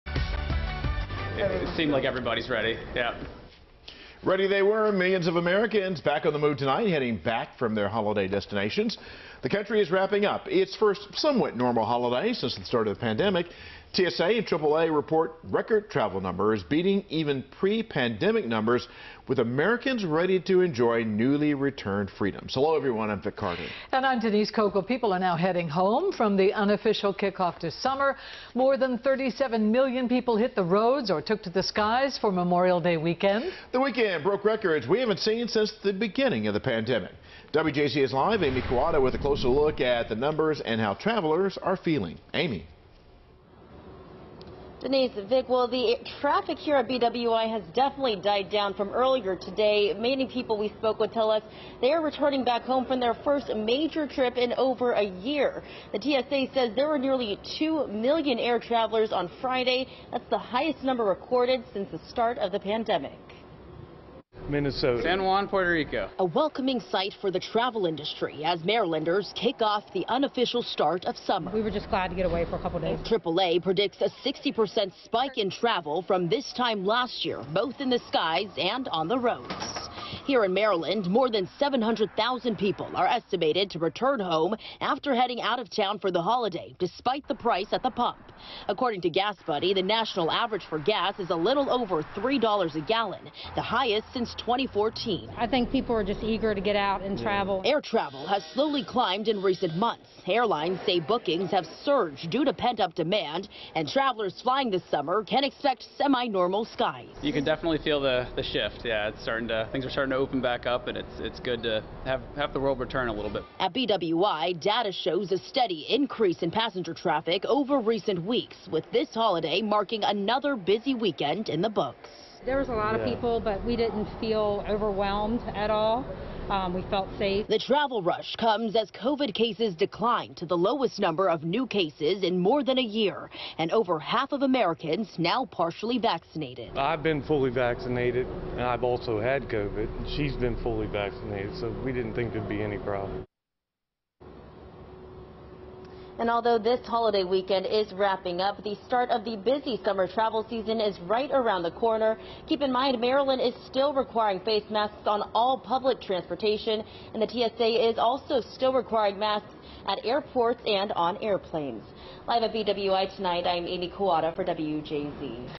More Than 700K Marylanders Expected To Return Home After Memorial Day Weekend
This Memorial Day weekend broke travel records that the U.S. hasn't seen since the start of the pandemic. More than 700,000 Marylanders are expected to return after heading out of town for the holiday.